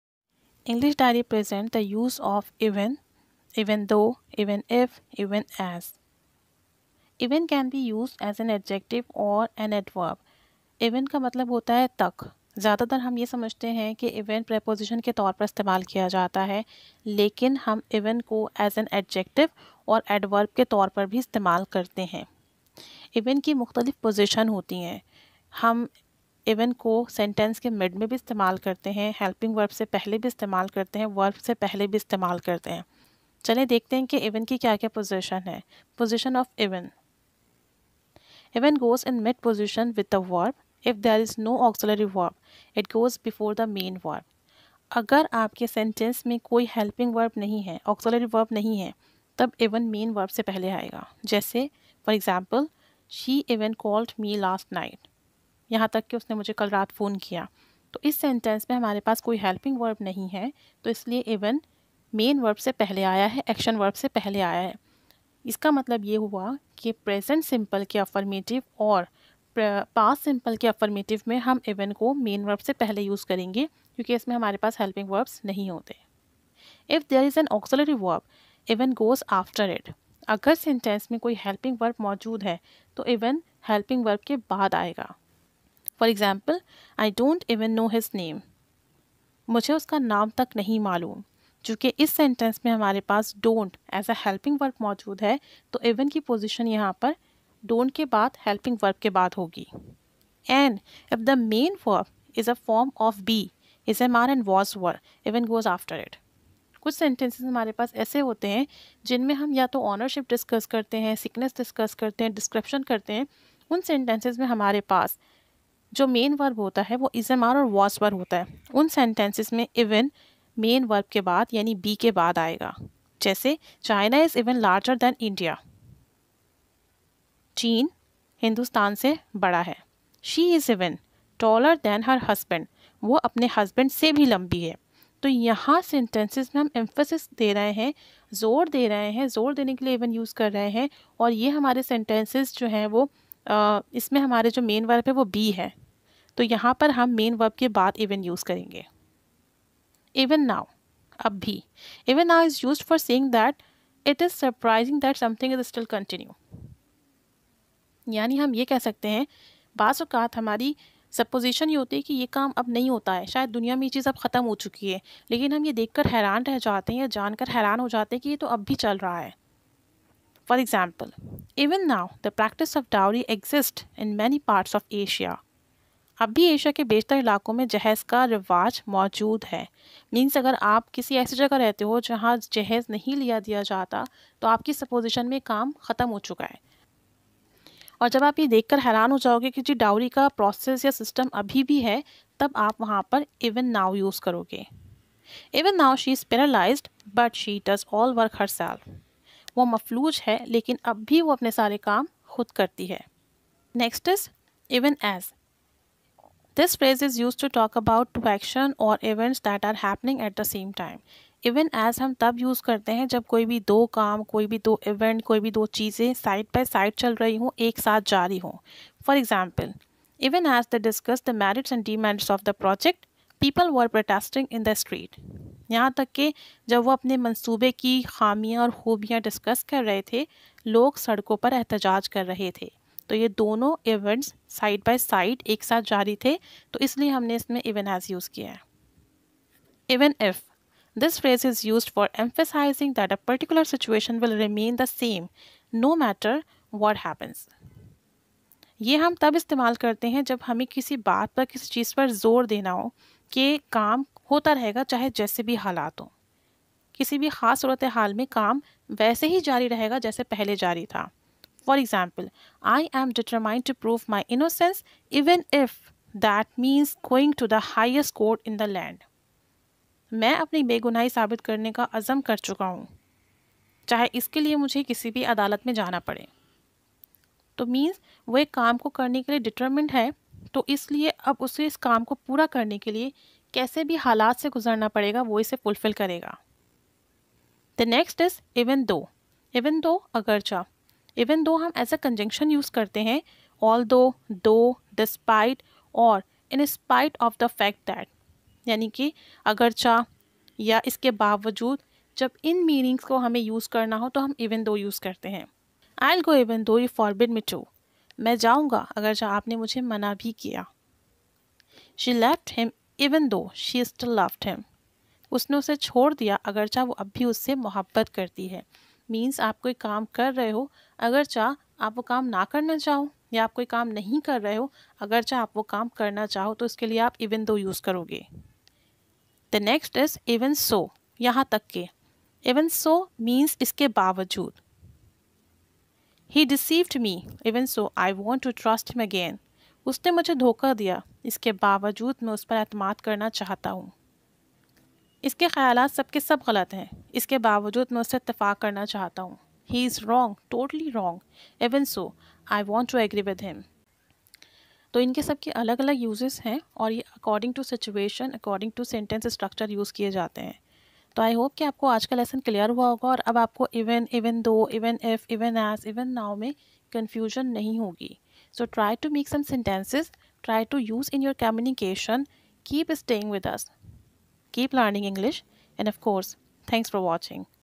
use of 'even though, even if, even as and even with | with Urdu meaning and examples
How to use 'even though, even if, even with, and even as' with Urdu meaning and examples. Learn all the uses and speak fluent English.
English diary